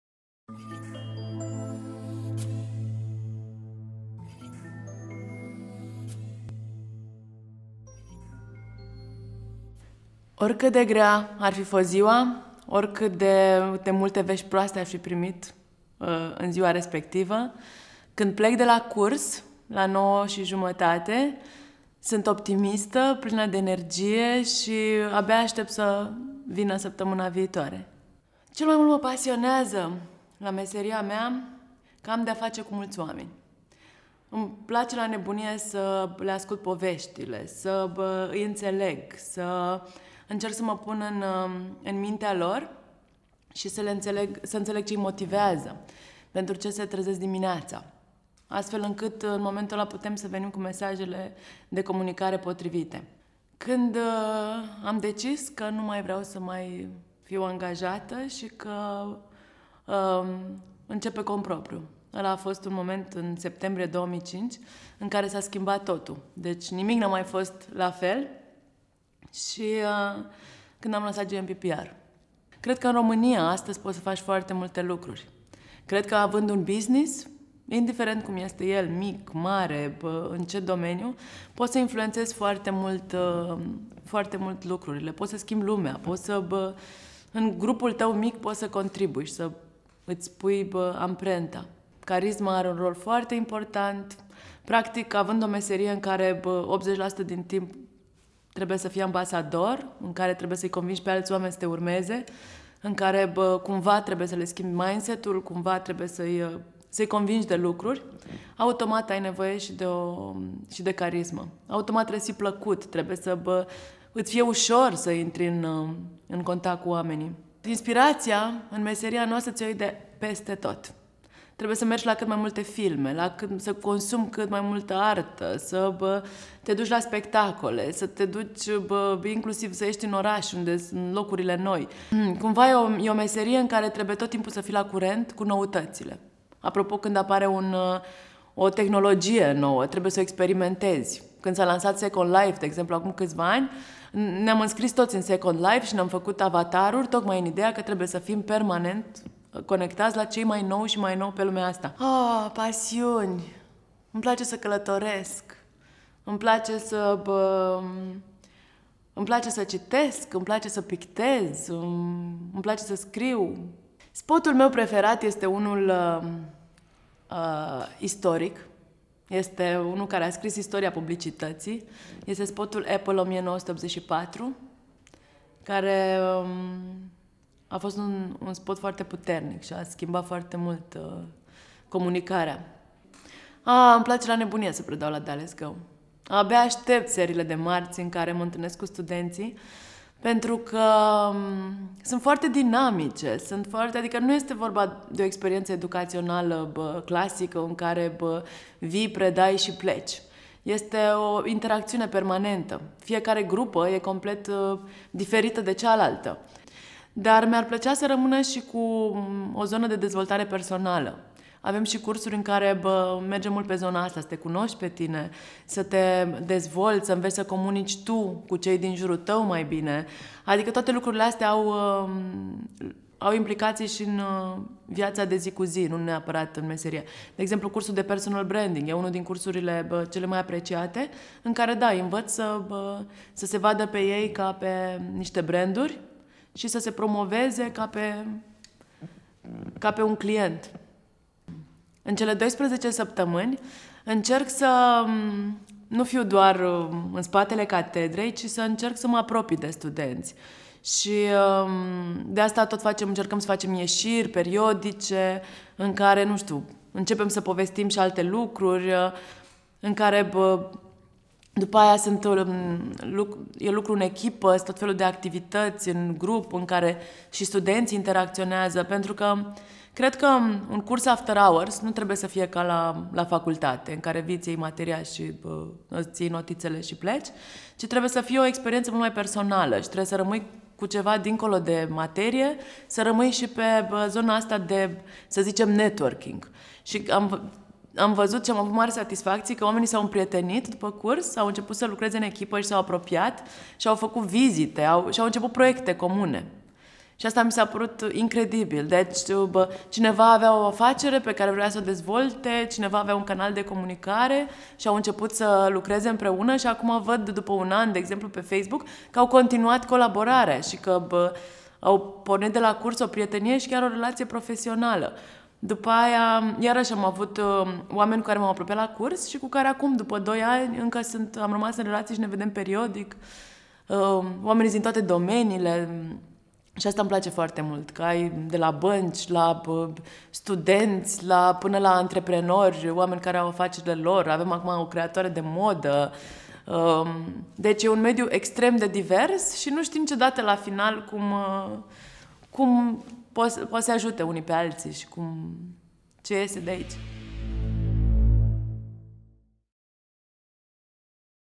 Ioana Manoiu
Cursurile ma umplu de energie, optimism si bucurie, mereu astept cu entuziasm sa vina saptamana urmatoare. Cel mai pasionant lucru la meseria de PR este, pentru mine, faptul ca intalnesc multi oameni, ca pot sa le ascult povestile si sa inteleg ce ii motiveaza, de ce se trezesc dimineata -- astfel pot croi pentru ei mesajele de comunicare potrivite. - Ioana Manoiu, DallesGO PR